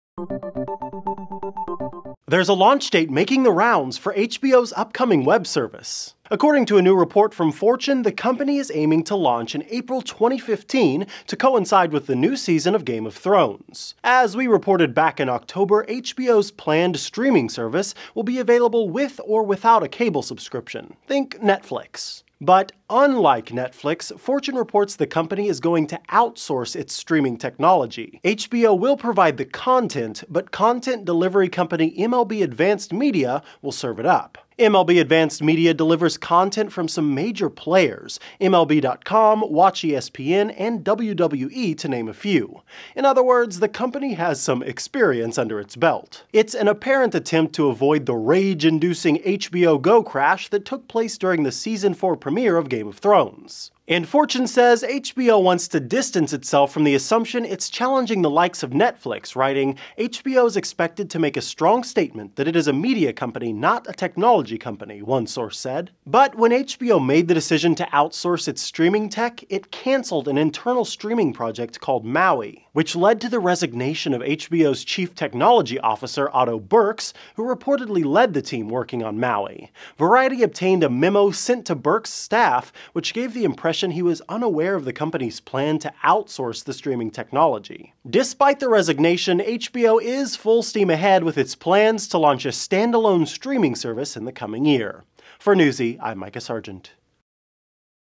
HBO's Stand-Alone Streaming Service Targeted For April
As HBO announced plans to outsource the technology behind its upcoming streaming service, its CTO announced his resignation.